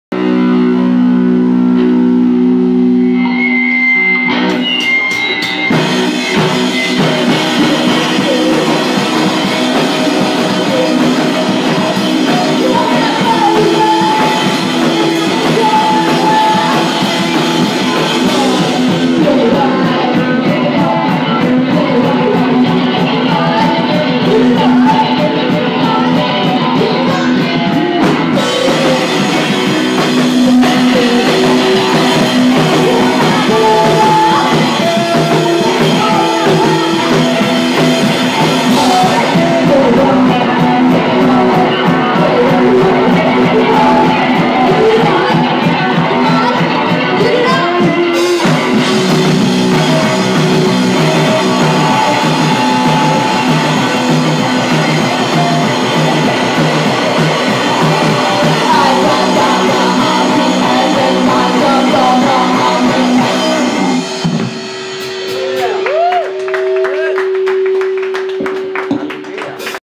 The Hussy at Quarters Rock and Roll Palace in Milwaukee, WI 2
The Hussy from Madison, WI playing live at Quarters Rock and Roll Palace in Milwaukee, WI on October 3, 2011.